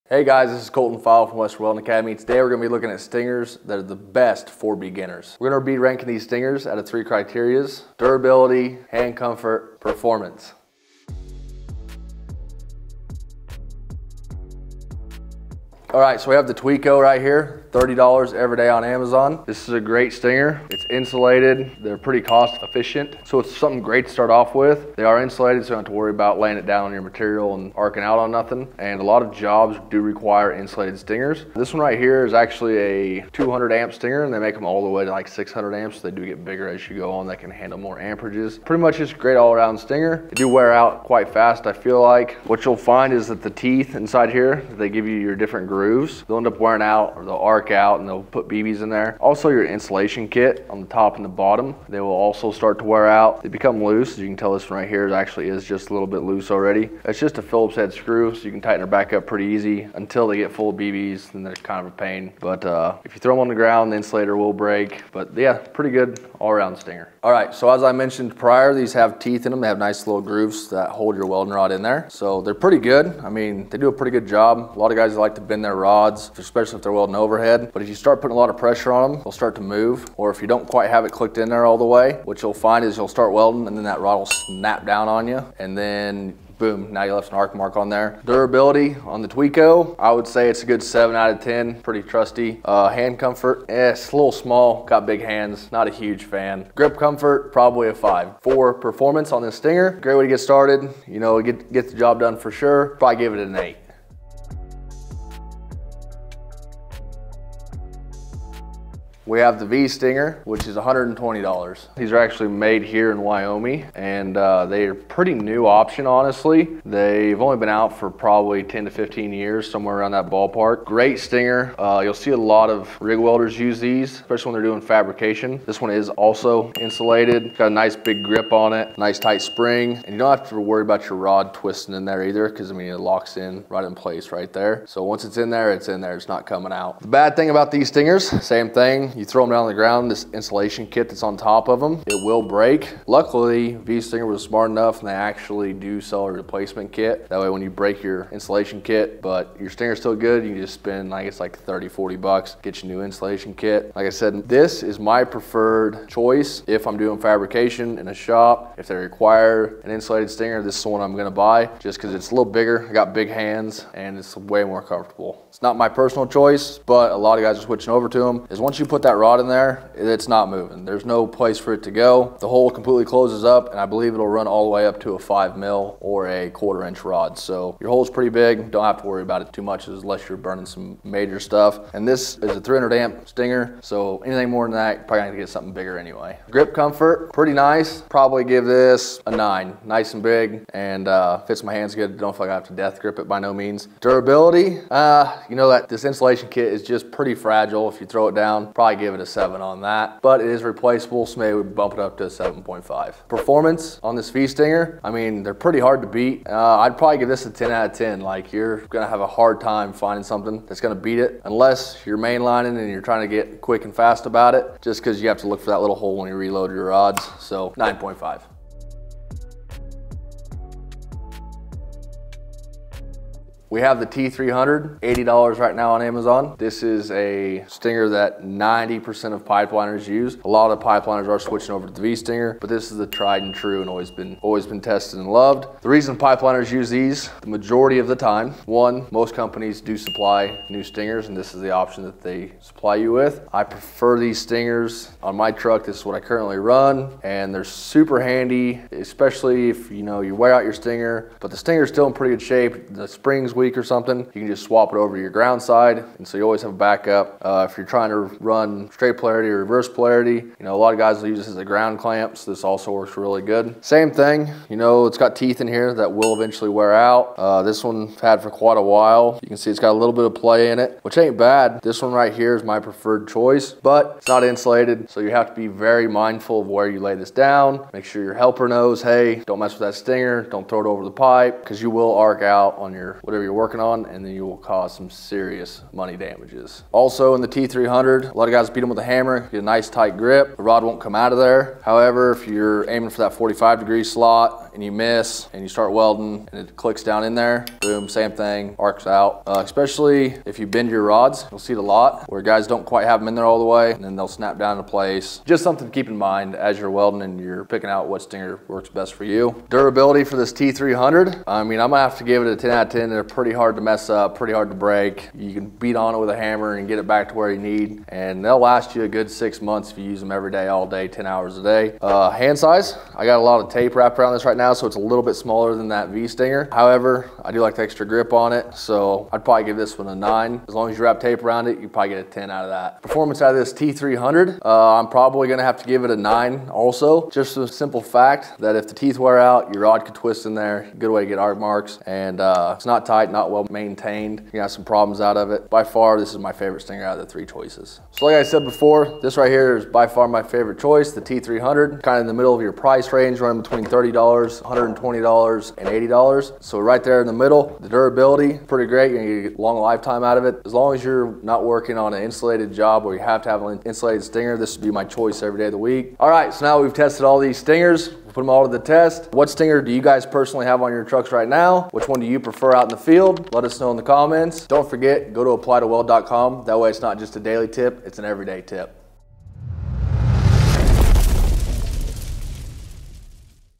The BEST Welding Stinger for BEGINNERS
In today's video, Western Welding Academy Instructor, Colton Pfeil goes over 3 different welding stingers compares them, rates them, and chooses which of the 3 he recommends the most for beginners.